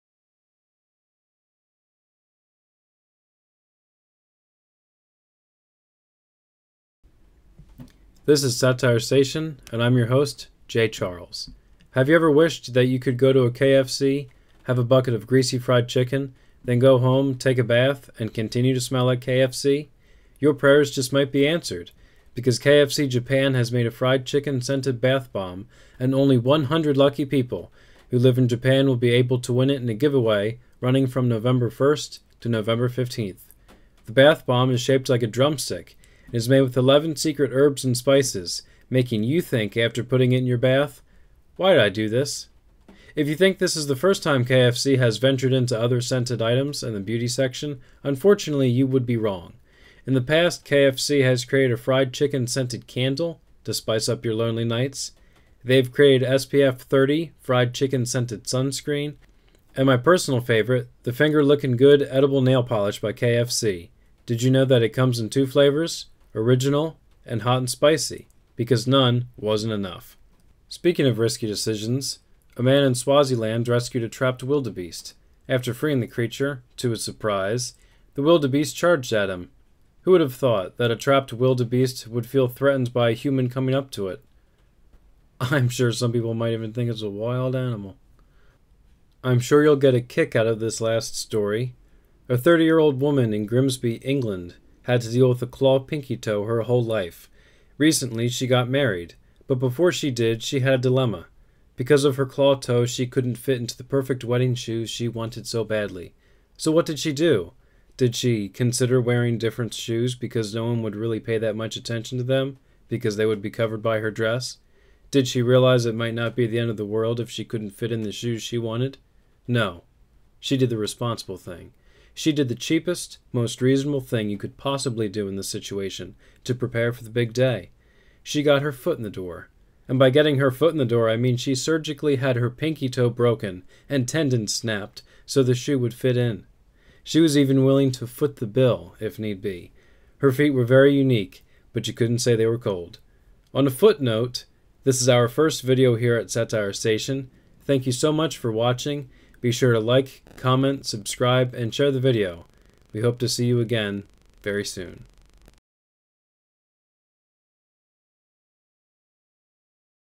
Chicken of the Bathtub - 4K News
Welcome to our first video! My name is Jay Charles and I report on news from all around planet Earth from my ship Satire Station! We hope you enjoy what you see.